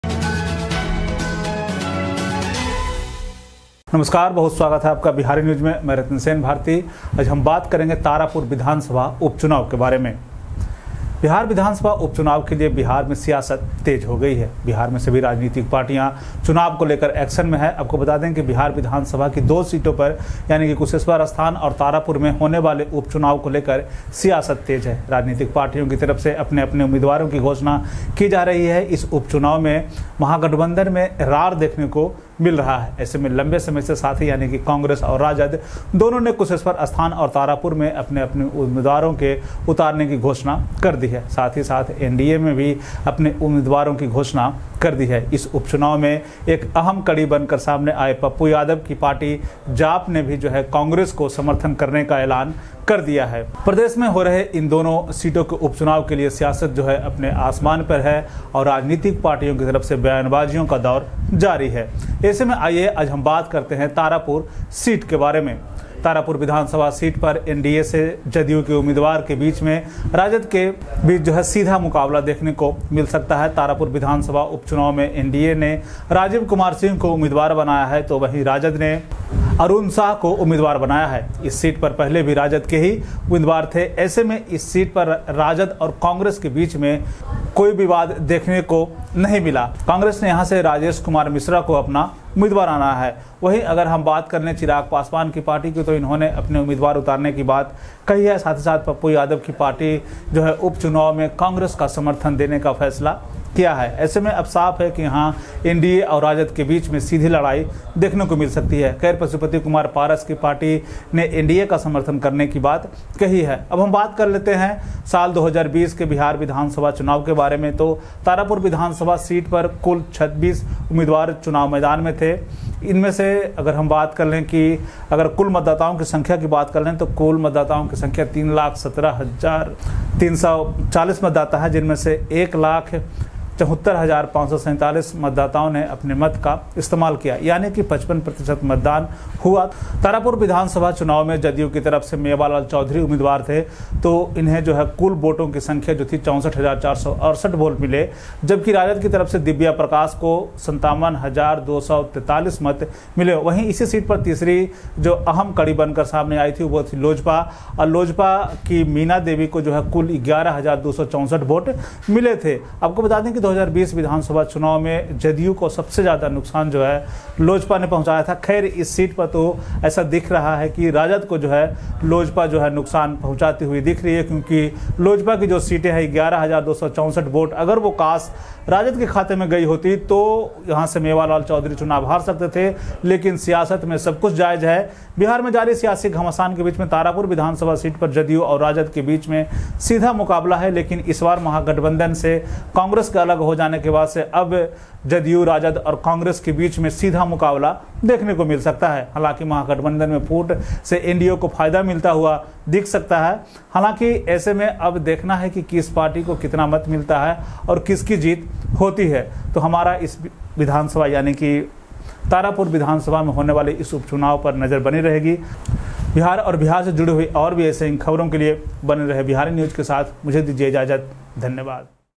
तारापुर विधानसभा उपचुनावः कौन बनेगा विधायक
Politics is intensifying in Bihar for the Bihar assembly by-election. All political parties in Bihar are in action regarding the elections. Let us tell you that the politics is intensified regarding the by-elections to be held in Kusheshwarsthan and Tarapur, two seats of the Bihar Legislative Assembly. Political parties are announcing their candidates. In this by-election, there is a ruckus in the Grand Alliance. In such a situation, both Congress and RJD have announced their candidates in Kusheshwar place and Tarapur for a long time. Simultaneously, NDA has announced its candidates. Pappu Yadav's party has emerged as an important link in this by-election, Jaap has also said to field its candidates in both these seats. In such a situation, politics is on its sky for the by-elections of these two seats in the state.